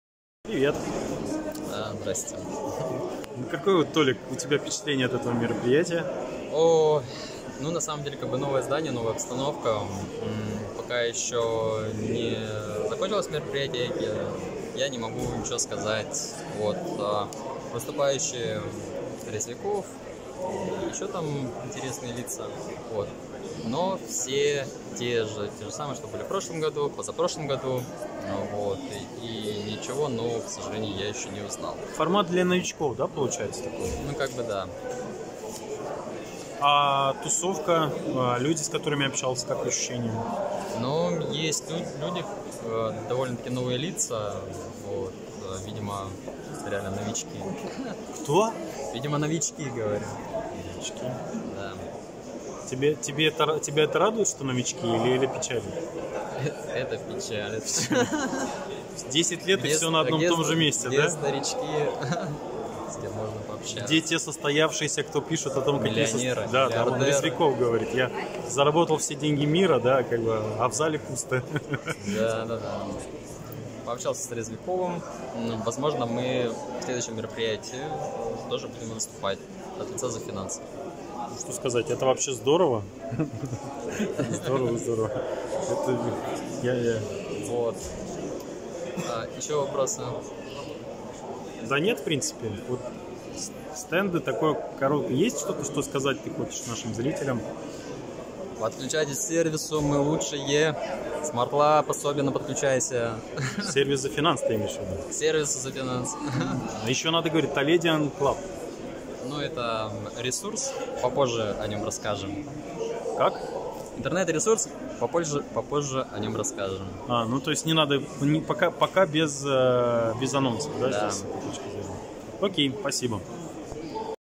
Впечатления о дне Петербургского инвестора, от участников и гостей.
2 часть. 1 июня 2019 года.